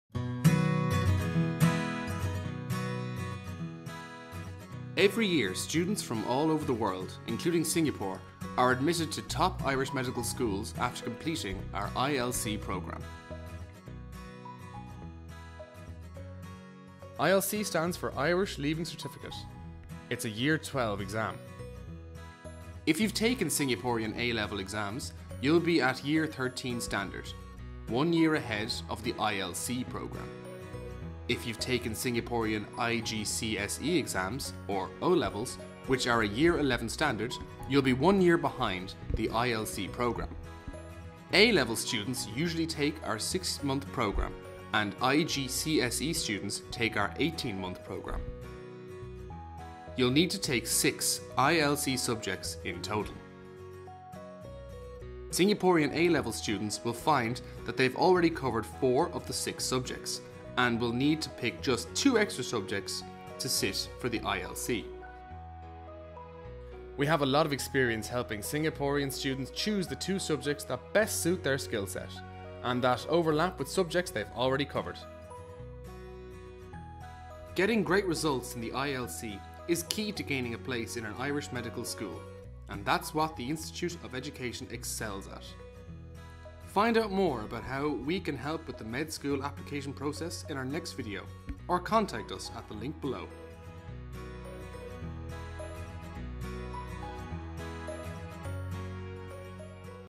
1. Study Medicine in Ireland (Singapore Students): What is the ILC?
Are you a Singaporean student looking for the best route to study medicine?

Find out more about the ILC programme in this short video.